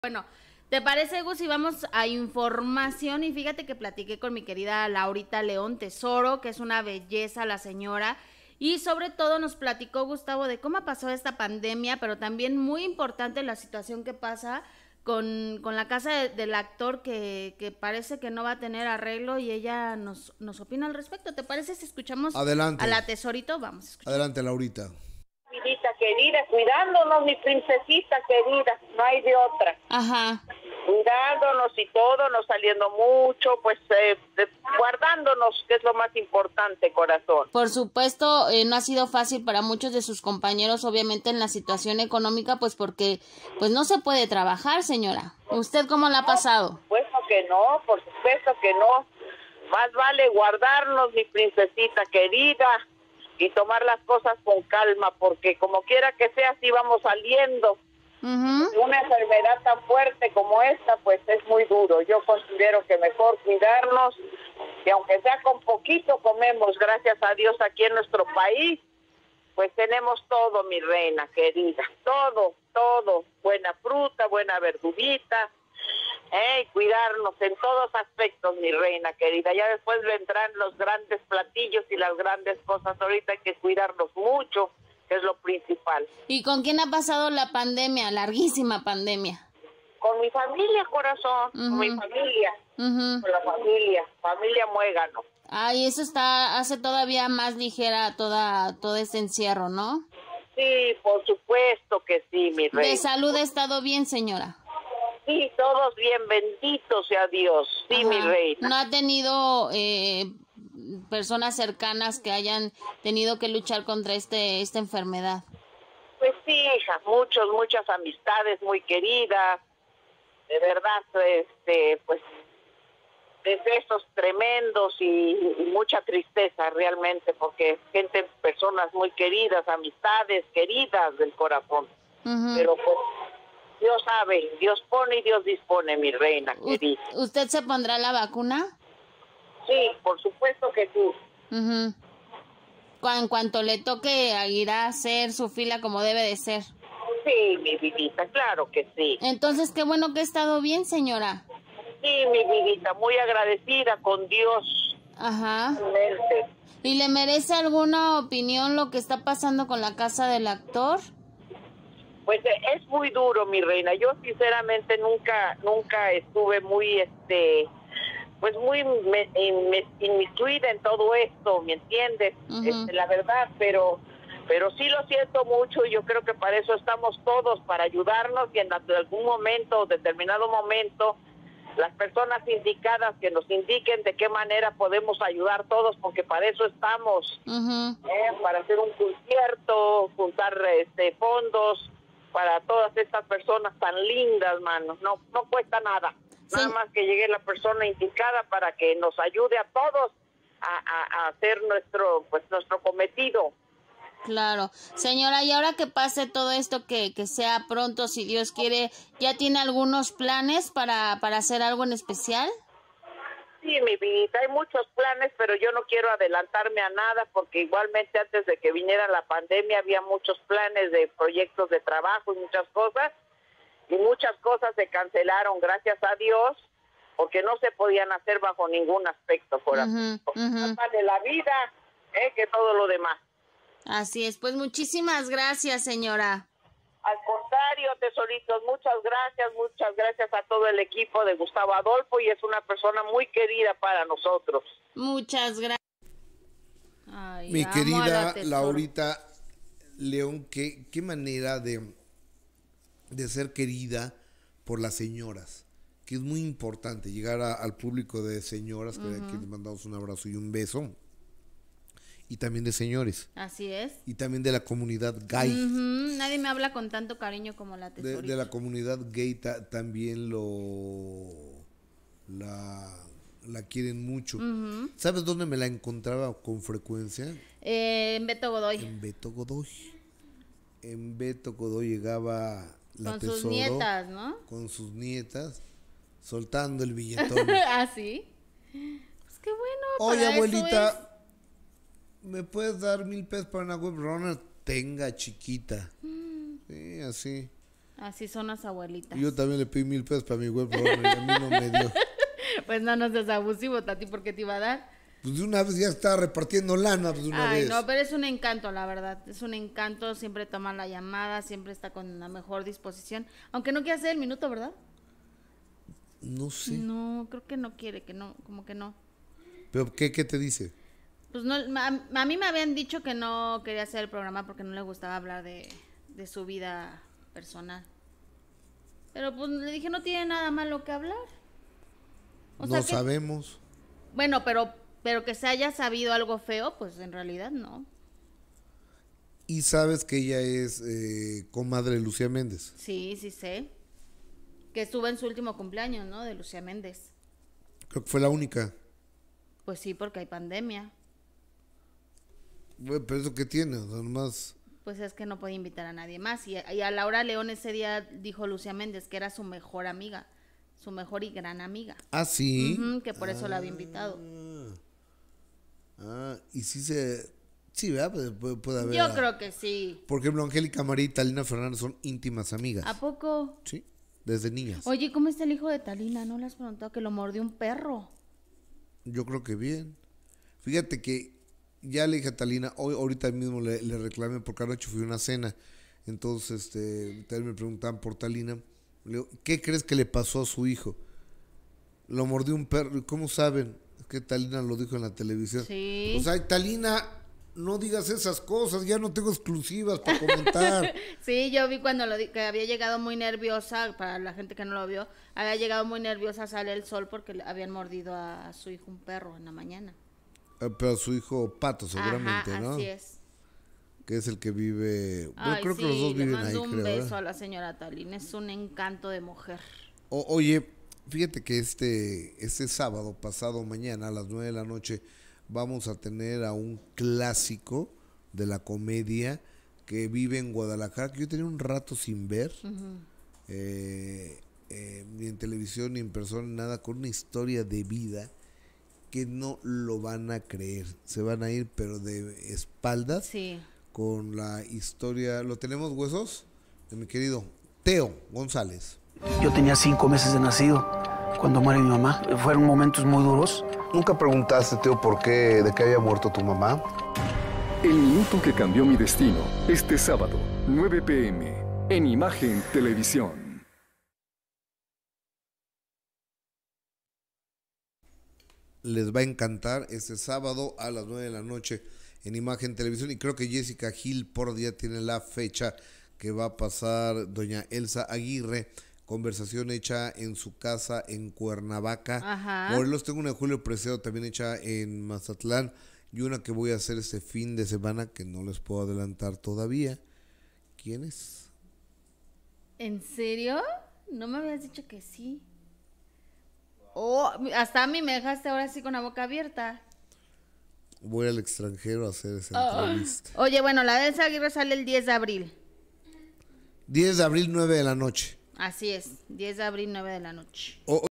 ¡Laura León invita a todos a unirse para salvar La Casa del Actor!
LauraLeón LaTesorito nos cuenta que ya quiere ponerse la vacuna contra el COVID19 e invita a todos a unirse para salvar